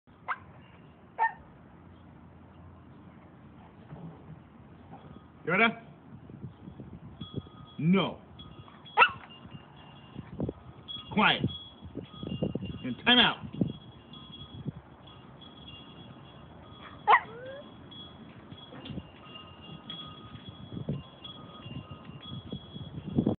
Yoda in timeout LOL
There he is in the cage for pooping on the heater.